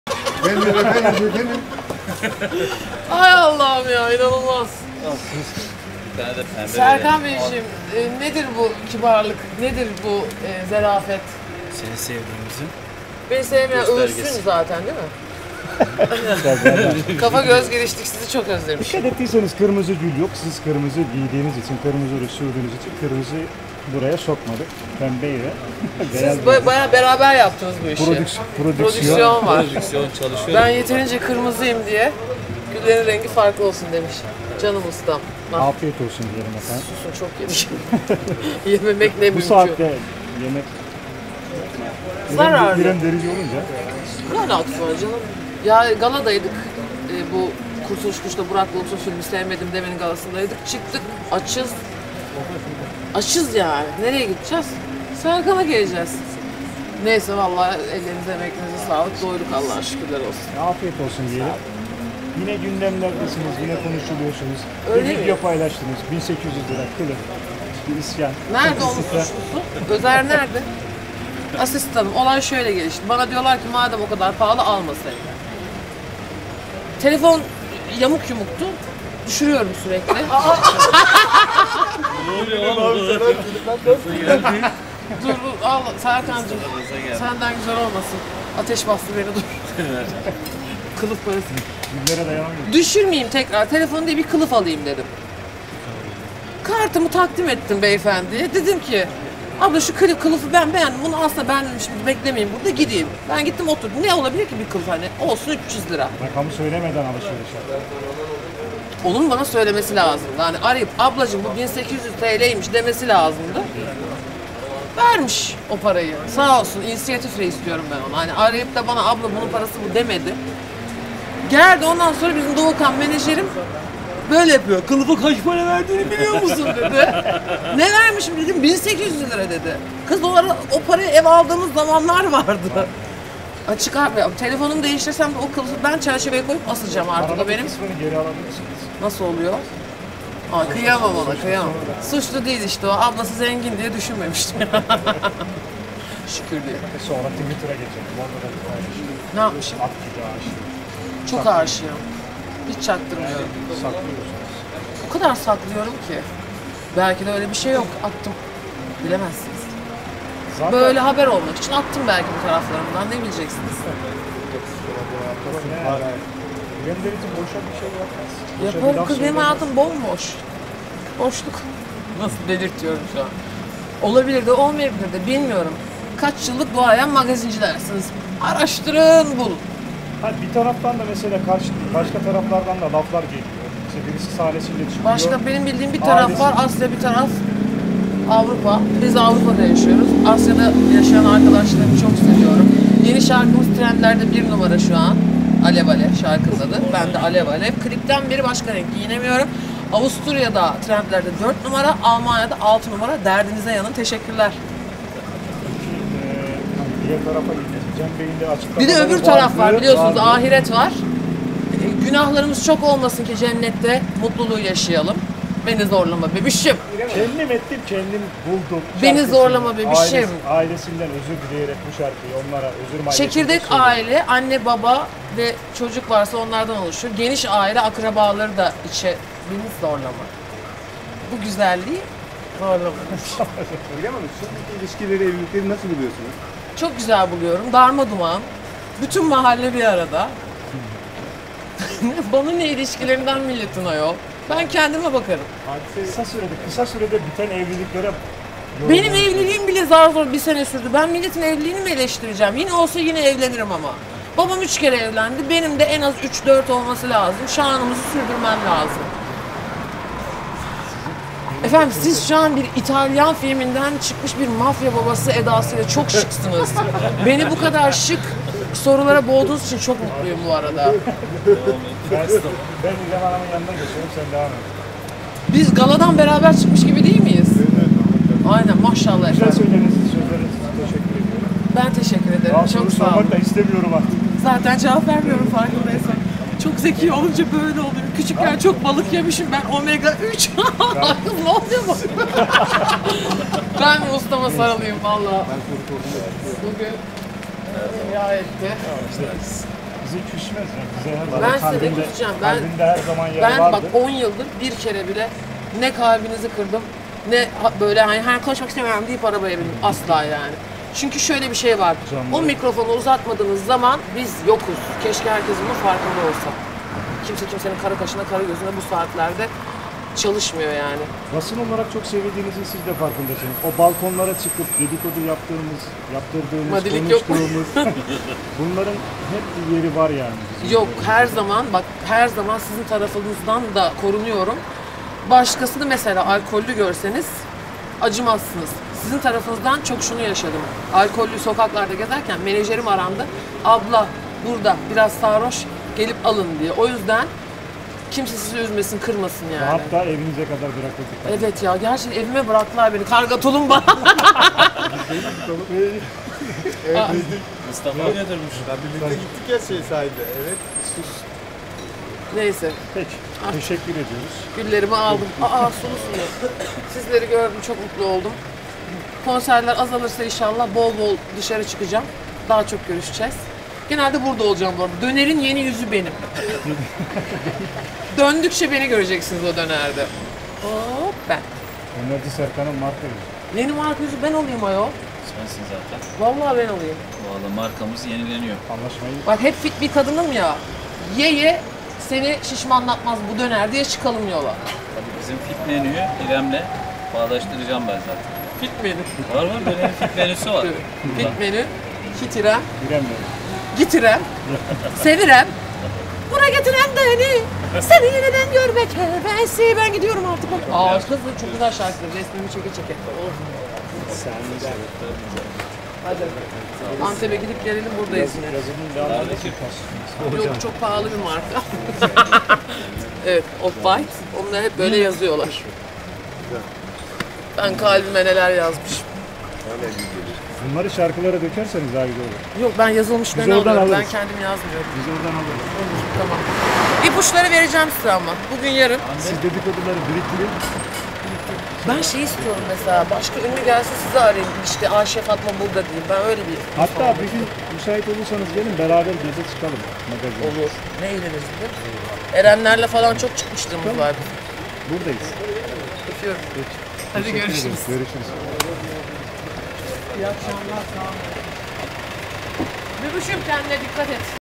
İrem Derici, ailesinden özür dileyen Cem Belevi'yle ilgili soruya ne yanıt verdi?
TÜRKSAT 4A
Frekans: 12729
Sembol: 30000
Polarizasyon: V (Dikey)